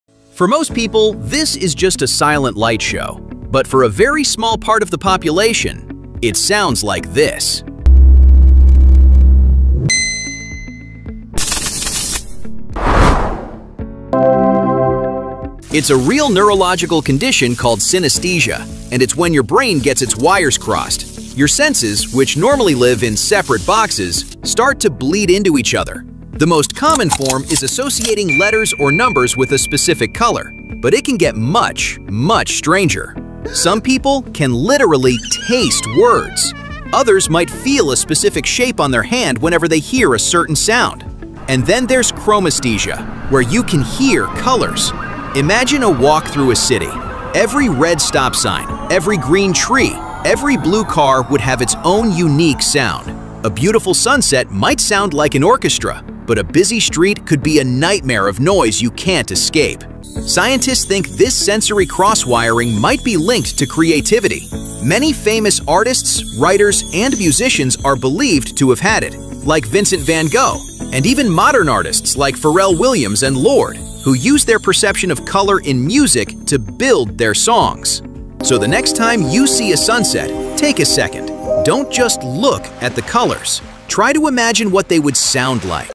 What if You Could Hear Colors?
What if you could hear colors? Imagine a world where music, voices, and everyday sounds don’t just echo — they paint the air with light. In this short, we explore synesthesia — specifically chromesthesia, where sound and color intertwine in the brain.

Some people naturally experience this sensory crossover. When they hear a note, a word, or a sound, they see colors. These “sound-color synesthetes” perceive a hidden spectrum in every melody.

Join us in imagining what your world might look like — if every sound had a shade, every voice a hue, every melody a painting. Drop a comment: What color do you think your favorite song would be?